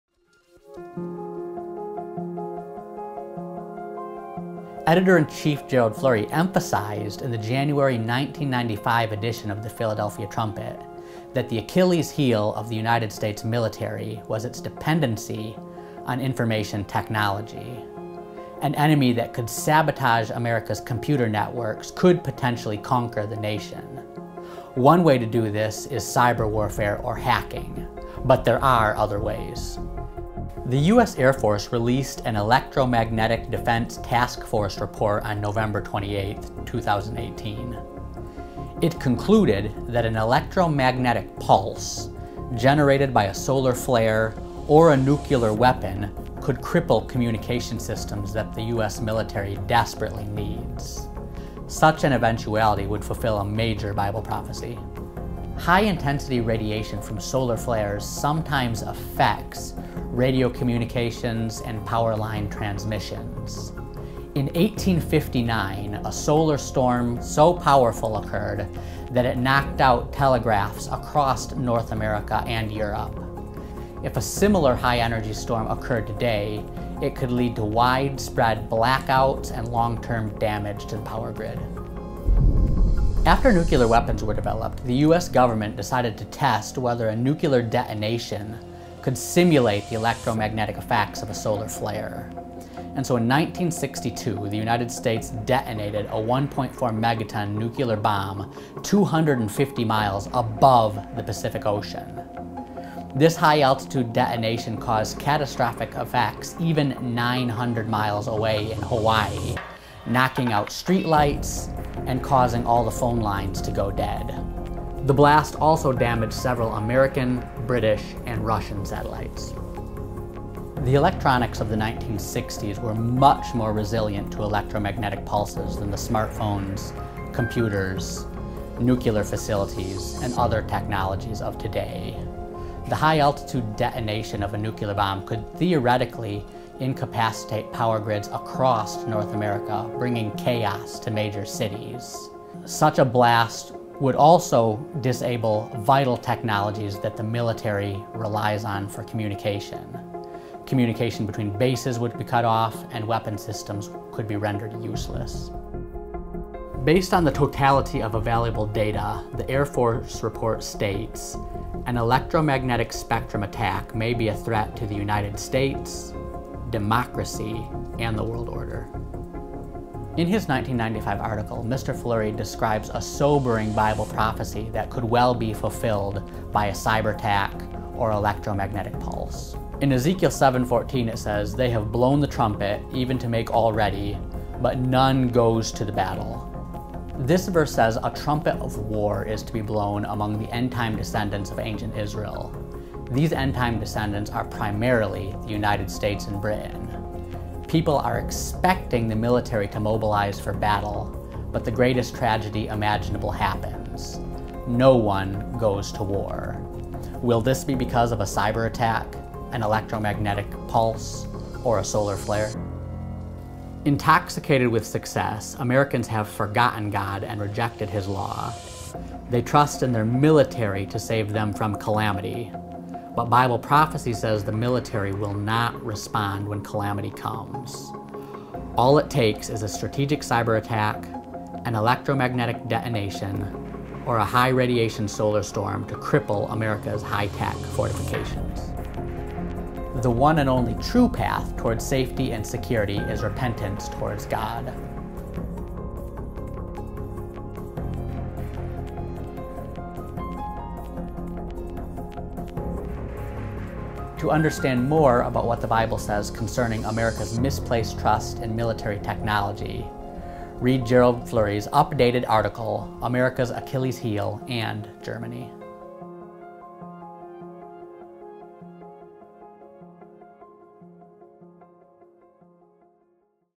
Electromagnetic Pulse Weapons Could Destroy U S Military
The world’s most powerful military has an Achilles’ heel.